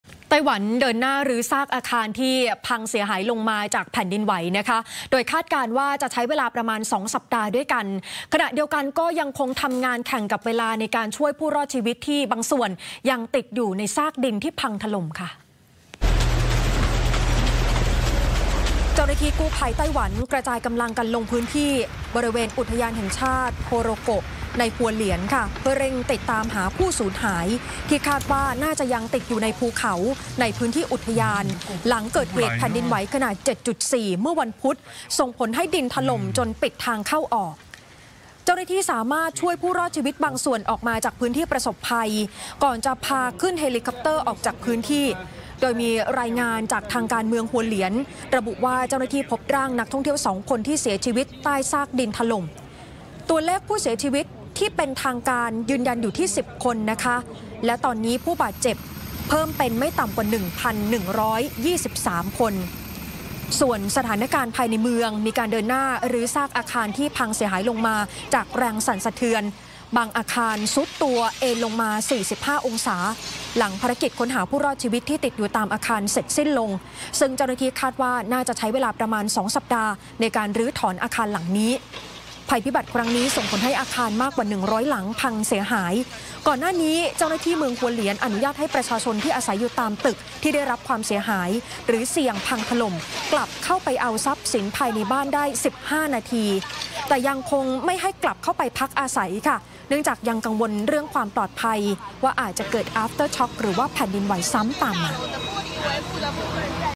"ไต้หวัน" เร่งช่วยผู้รอดชีวิตที่ติดเขา หลังแผ่นดินไหว l ข่าวค่ำมิติใหม่
"ไต้หวัน" เร่งช่วยผู้รอดชีวิตที่ติดเขา หลังแผ่นดินไหว